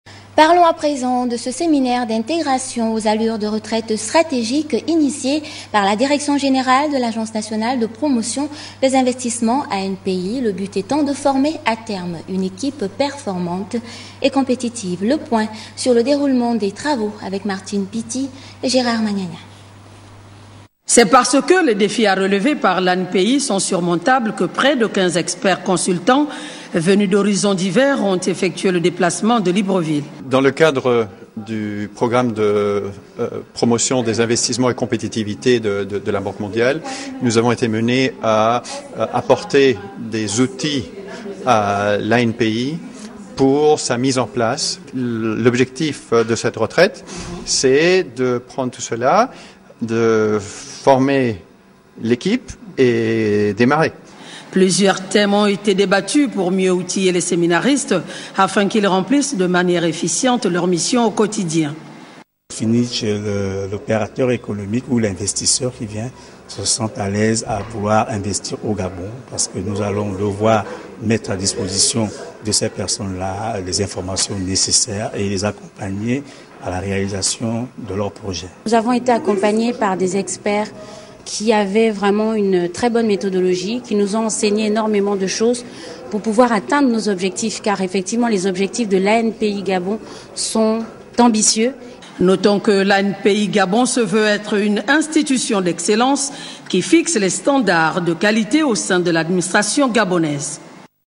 RTG / Séminaire d’intégration aux allures de retraites stratégiques initié par ANPI-Gabon
Séminaire d’intégration aux allures de retraites stratégiques initié par la Direction Générale de l’ANPI-Gabon